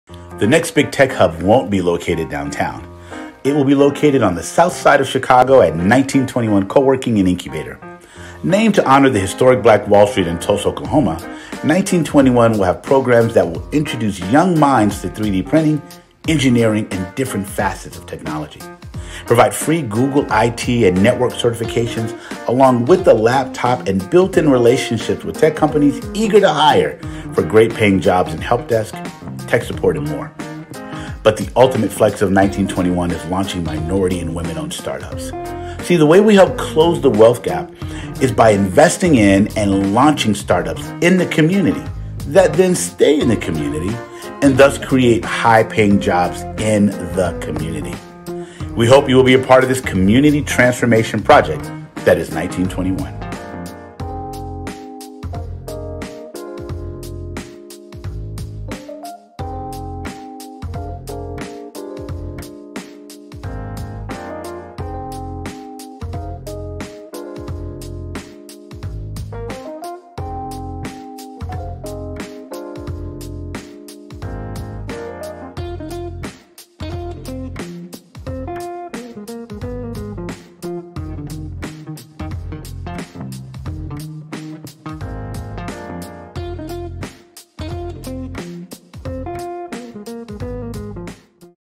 1921 Coworking and Incubator Funding Initiative Video
Hello. Thank you for taking a moment to view our 1921 Coworking and Incubator Funding Initiative Video. If you’d like to be a corporate sponsor or make a donation to help us run programs focused kn Young Tech, IT Certifications, and Launching Tech Startups on the Southside of Chicago, please or you can make a donation via $donate1921